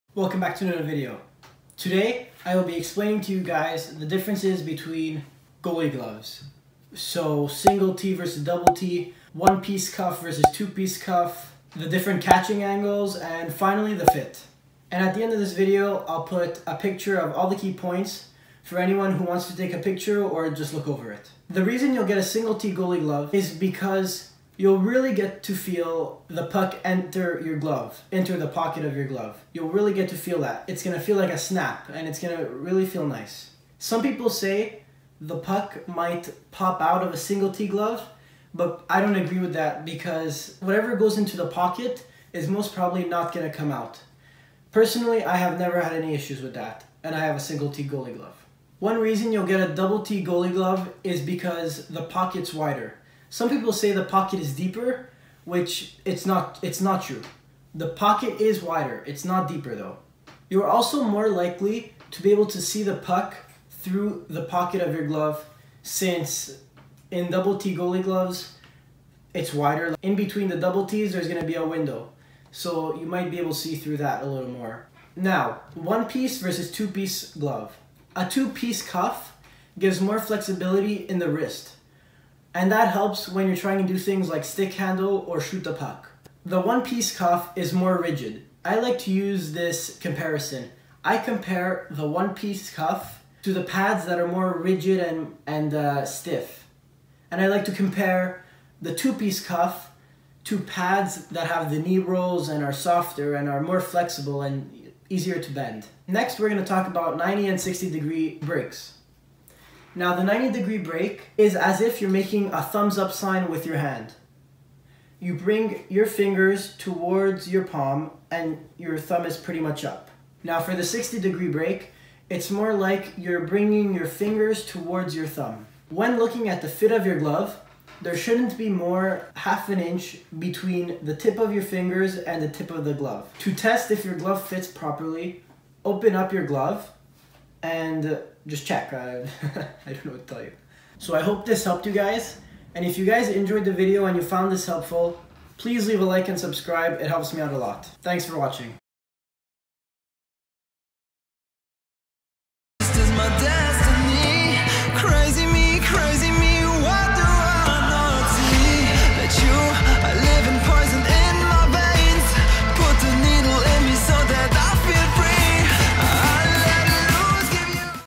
Different Types of Hockey Goalie Gloves | EXPLAINED!
Single T vs Double T: 0:21
1 Piece vs 2 Piece Cuff: 1:14
90 vs 60 Degree Breaks: 1:46
Fit: 2:05
Key notes: 2:28